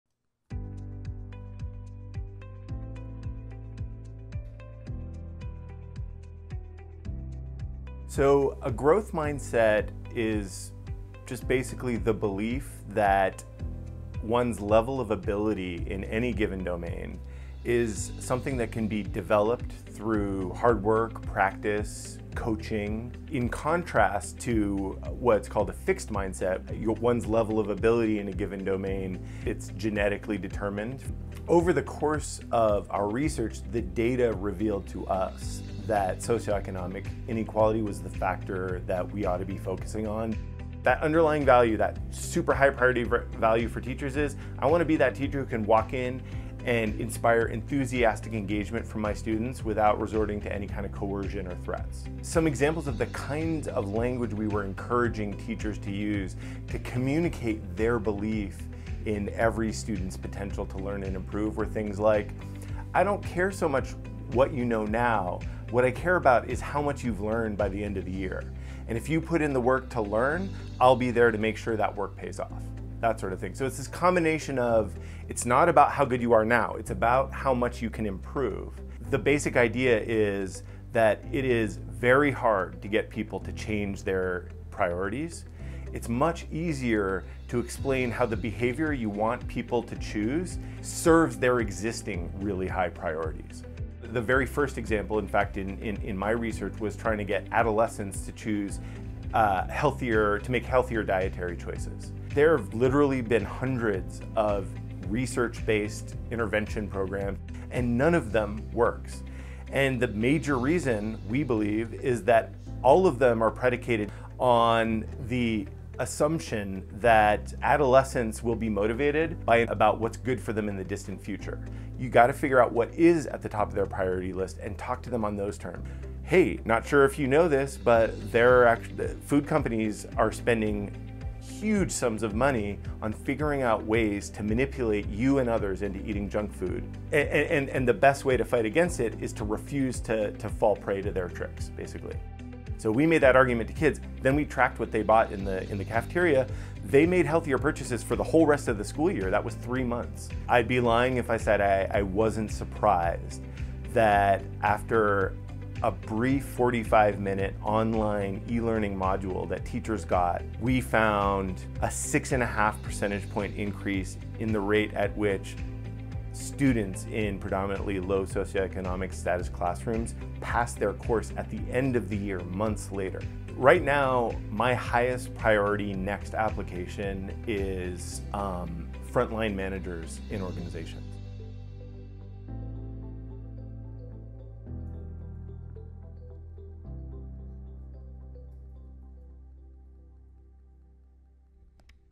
Growth Mindset Teaching Helps Students Make the Grade
By focusing on the potential to improve, teachers improve pass rates, especially for teens from low socioeconomic backgrounds